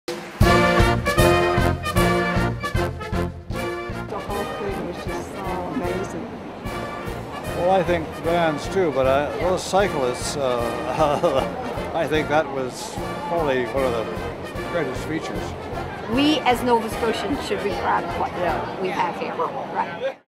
What the Audience Said...
We stopped a few audience members on their way out of the Metro Centre to get their thoughts on the show! Here's what they thought of the July 3, 2014 show.